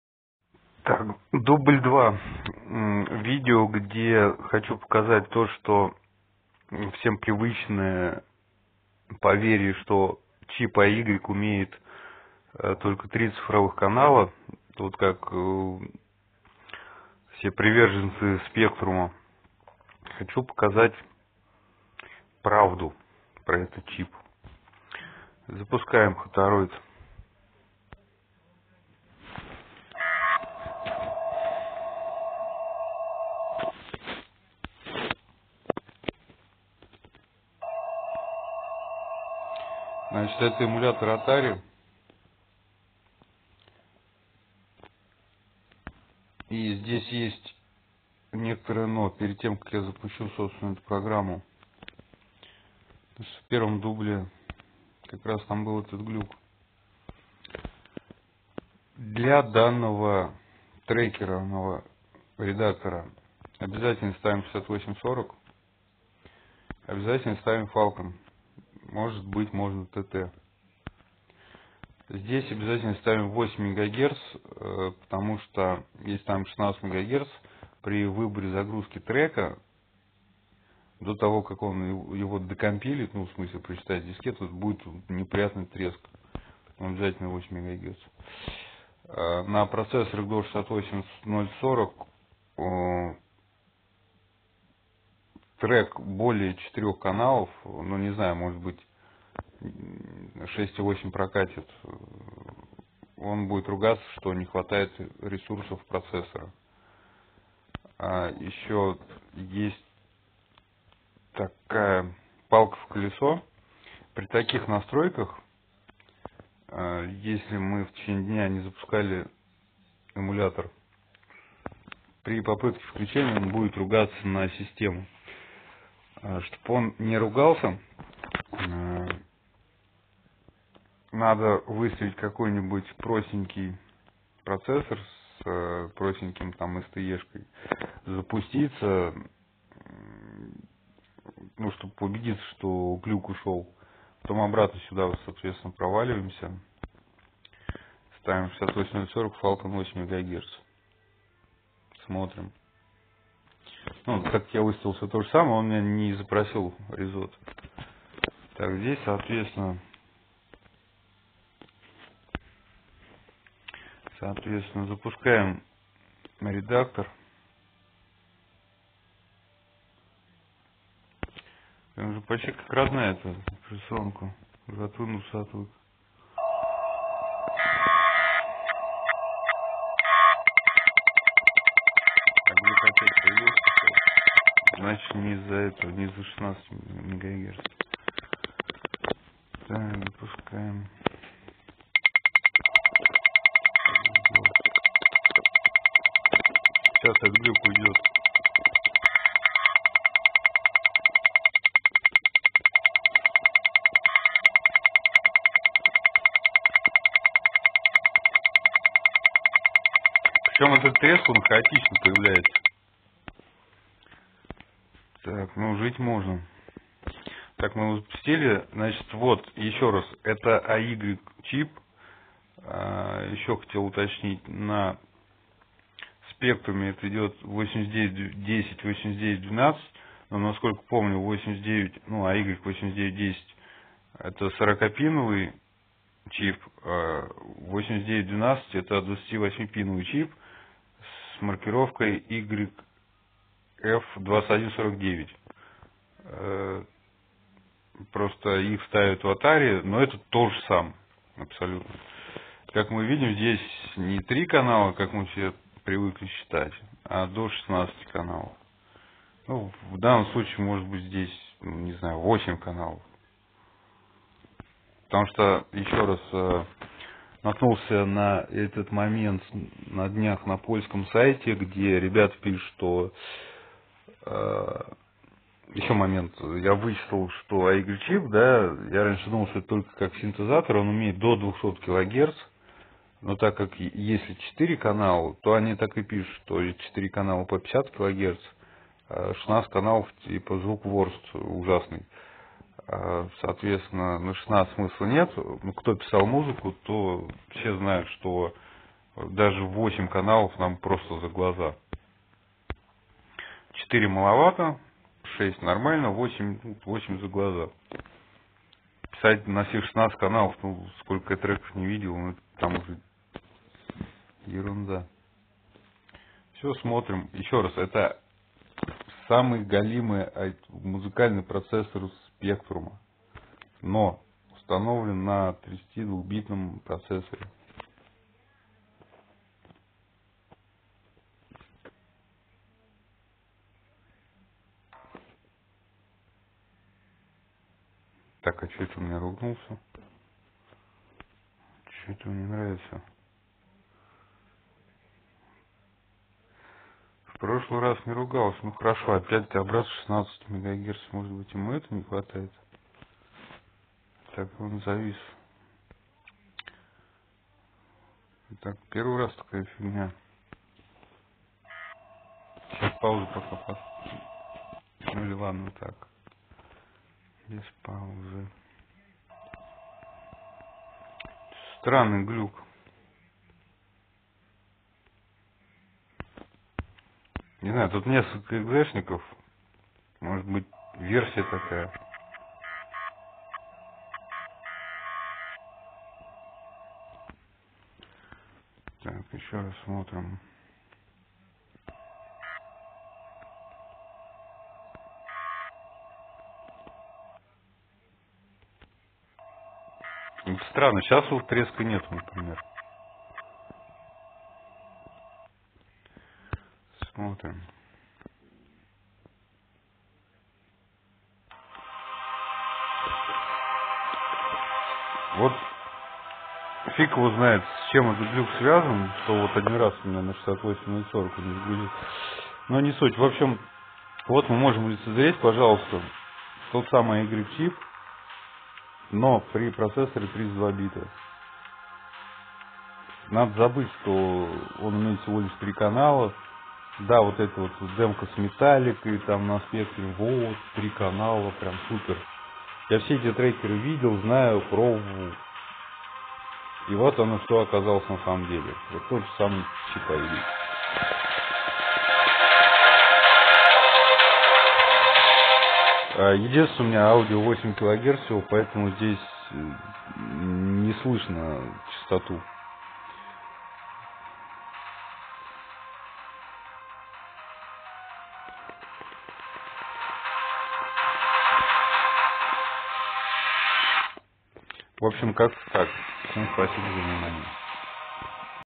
ATARI ST - HEX TRACKER PLAY 8ch track on AY PSG. Yamaha YM2149F up to 16 channels! It's REALLY
Ваша поддержка на патреоне- двигатель прогресса.

Если кто думает что звуковой чип "спектрума умеет всего ТРИ четырех битных цифроканала, то ошибается.